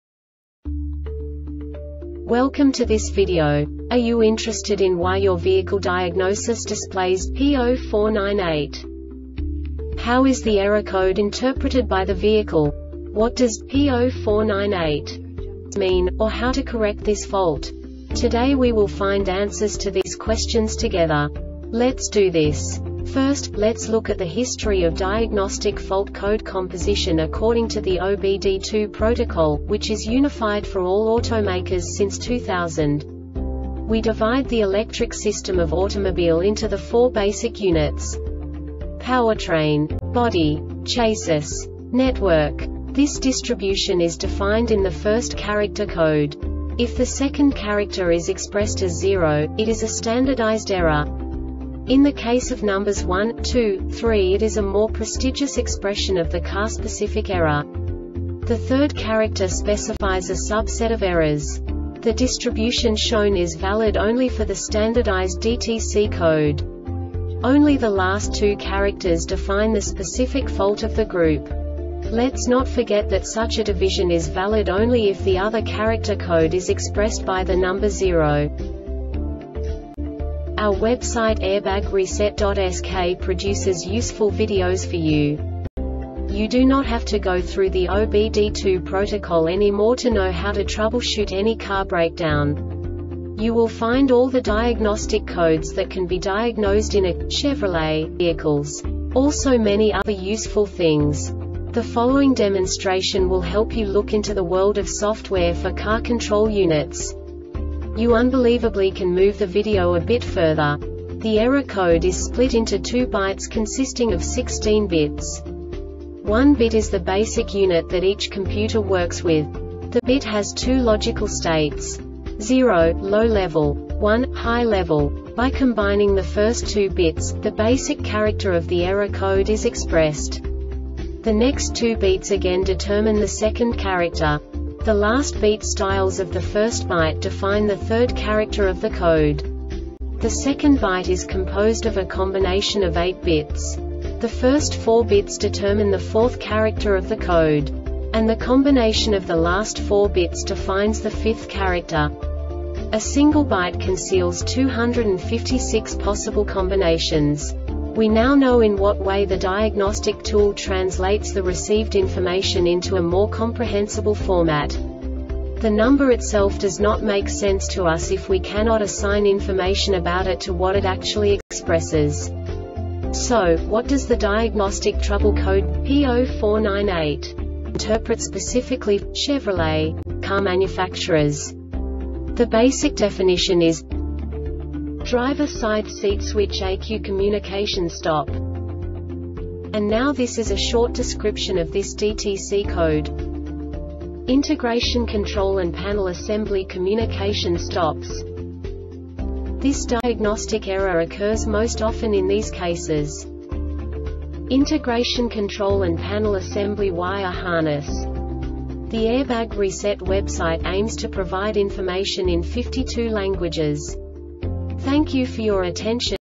DTC Chevrolet P0498 Short Explanation
Contents:
0:21 Basic DTC analysis according to OBD2 protocol standard.
1:48 Insight into programming
2:58 A diagnostically understandable description of the Diagnostic Trouble Code
3:05 Basic error definition

Make:
Chevrolet
Code:
P0498
Definition:
Driver Side Seat Switch ECU Communication Stop
Description:
Integration control and panel assembly communication stops
Cause:
 Integration Control and Panel Assembly Wire harness